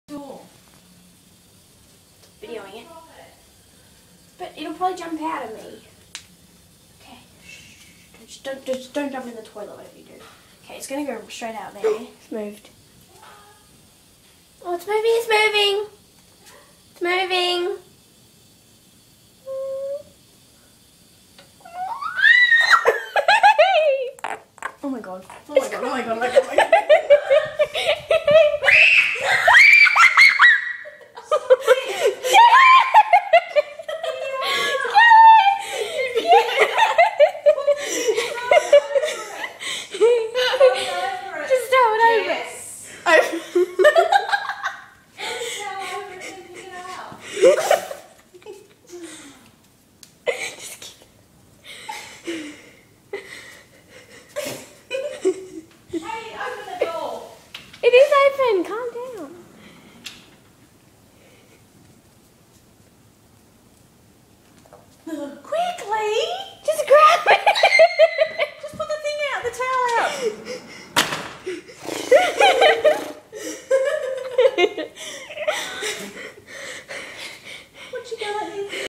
Frog on the toilet seat!!
How my brave daughter removed a vicious, girl eating green frog from the toilet. Now that's funny!!!